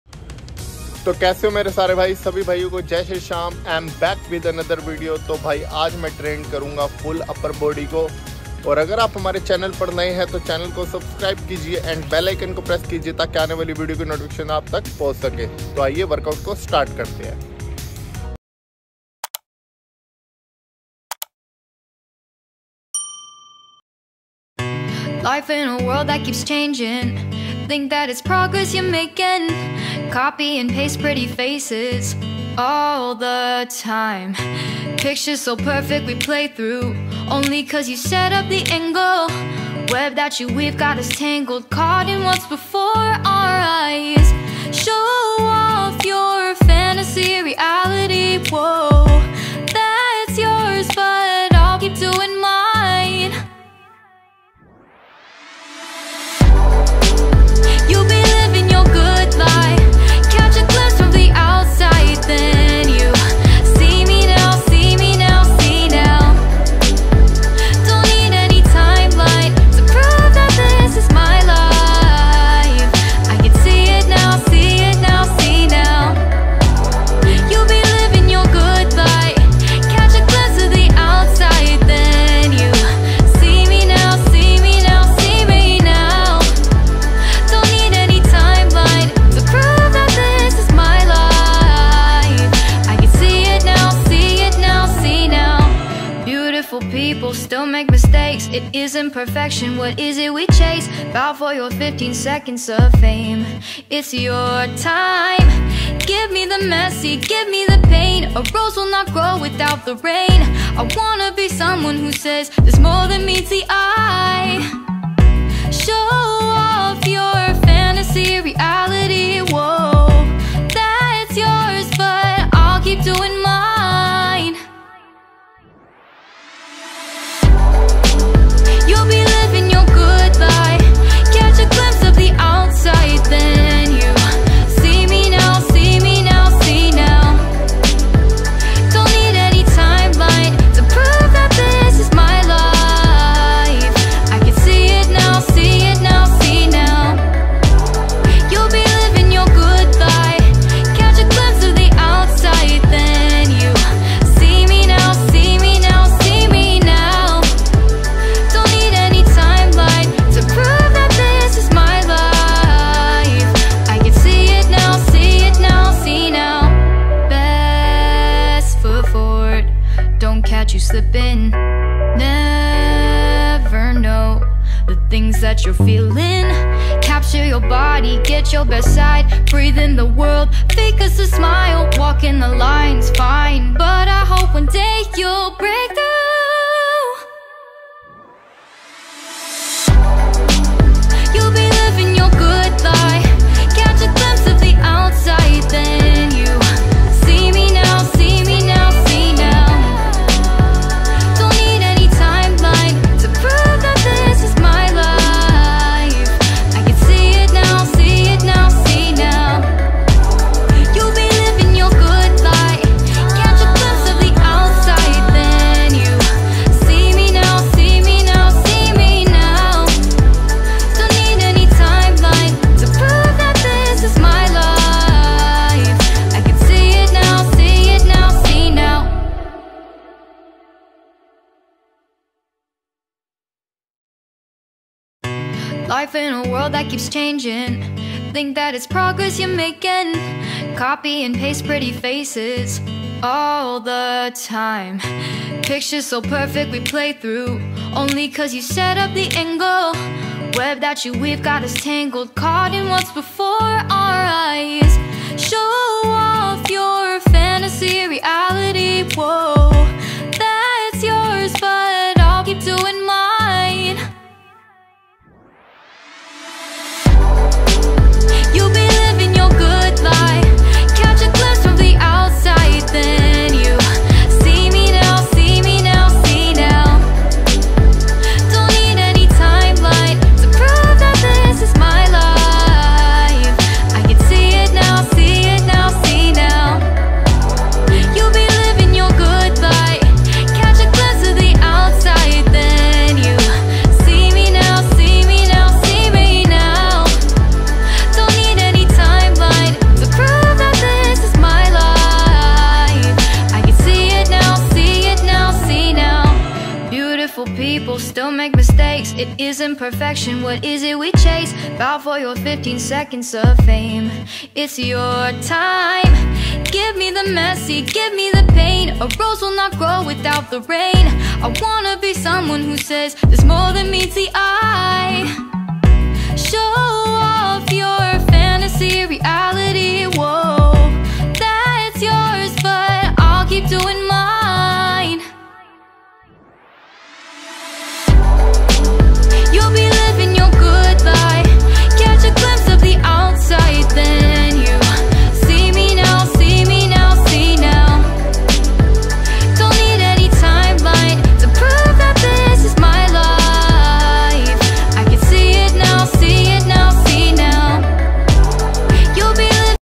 Full upper body workout||@jatinnfitnessseries8928
Jai shree shyam to all of you

Full upper body workout

1.flat bench press

2.cable crossover

3.shoulder press machine

4.lateral raise machine

5.cable rope shrug

6.pull down machine

7.seated row

8. Barbell biceps curl

9. V handle triceps Pushdown

10. Wrist curl